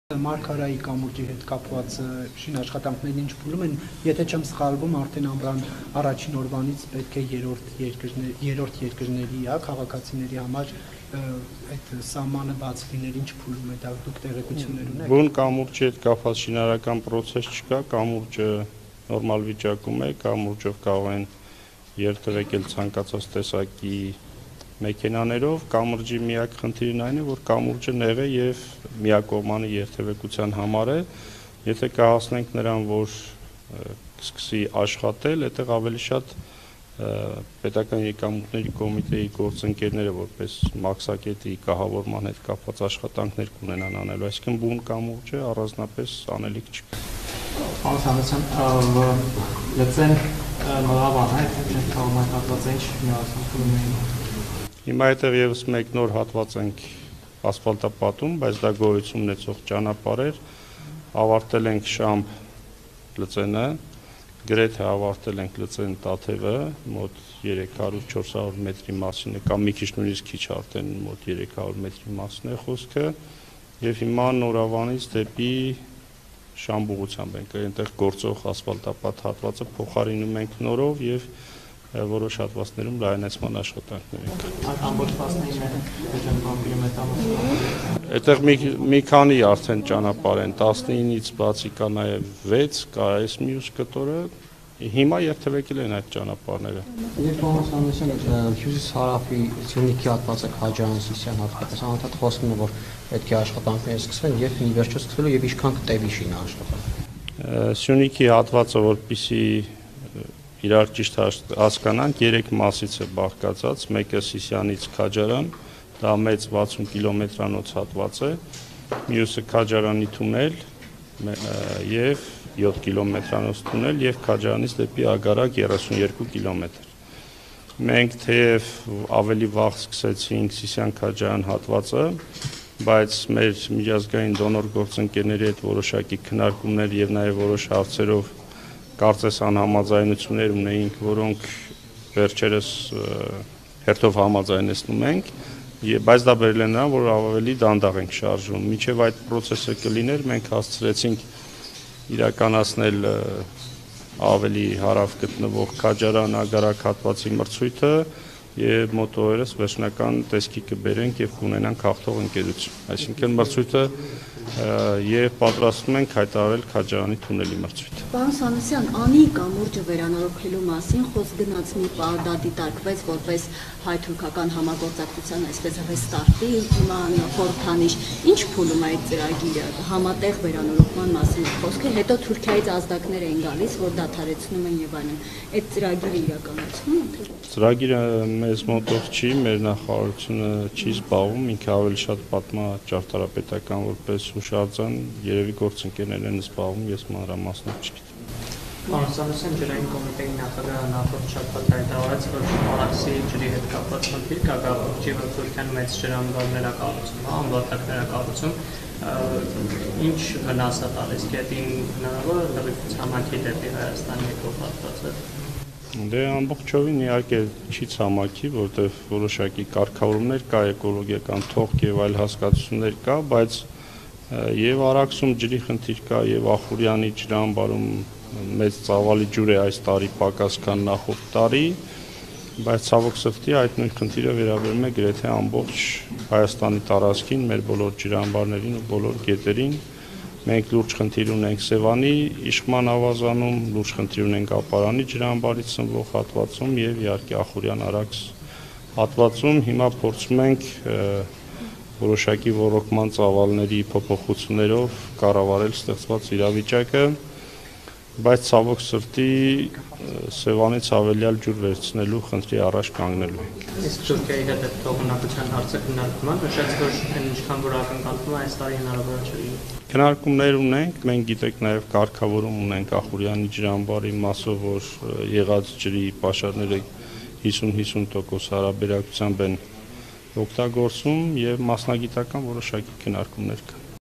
Մոտ օրերս կիմանանք Քաջարան-Ագարակ հատվածի մրցույթի հաղթողի անունը
Մոտ օրերս կիմանանք Քաջարան-Ագարակ հատվածի մրցույթի հաղթողի անունը. պատրաստվում ենք հայտարարել Քաջարանի թունելի մրցույթը. Սանոսյանը` Հյուսիս-հարավ ճանապարհի մասին